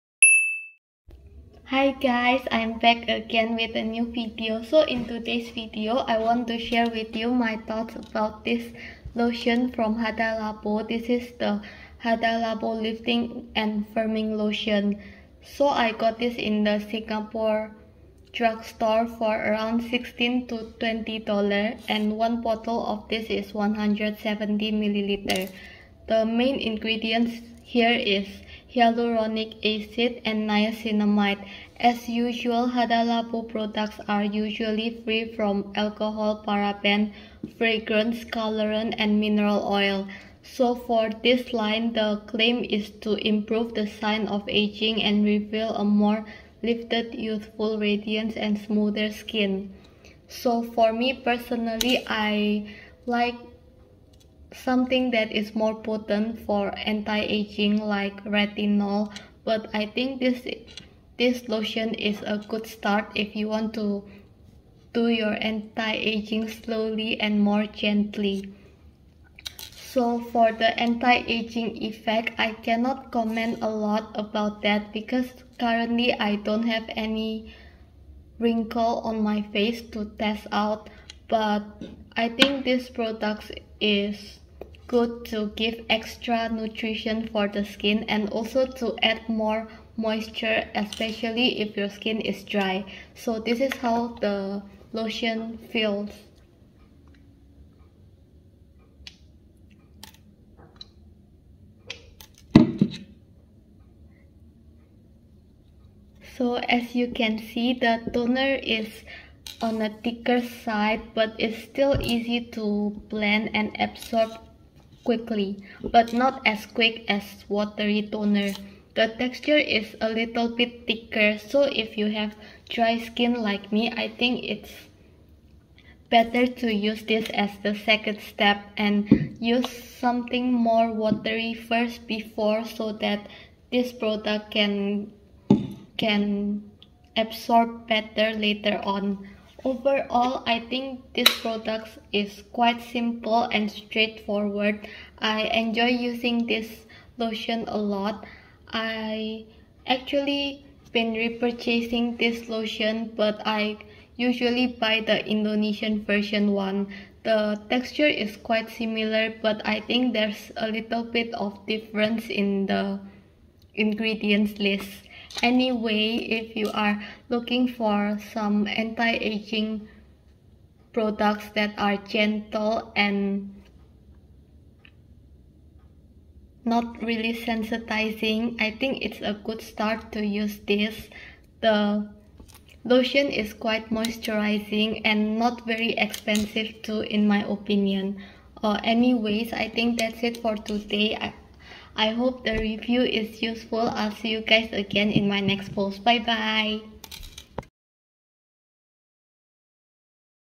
Hada Labo Lifting And Firming Lotion Review
I bought the hada labo aging care lotion a few months ago in Singapore drugstore. The lotion cost me around $16 to $20 for 170ml. In my opinion the price is quite normal for drugstore products. As usual hada labo products are usually free from parabens, alcohol, colorants and fragrances. The main ingredients in this lotion are hyaluronic acid and niacinamide.

The product claims are for anti aging and lifting. I personally like to use retinol more as it is more potent than this. Currently i cannot comment a lot about the anti aging effects as i dont have any wrinkles to test out. But i think this is a good start if you want to prevent wrinkles gently.

The texture of the lotion is thick unlike other toners out there. I personally like this kind of consistency as i have drier skin type. Overall i think this is a good products for preventions and adding more nourishnment for the skin. I have been a loyal hada labo fan and i definetly would buy this again and again.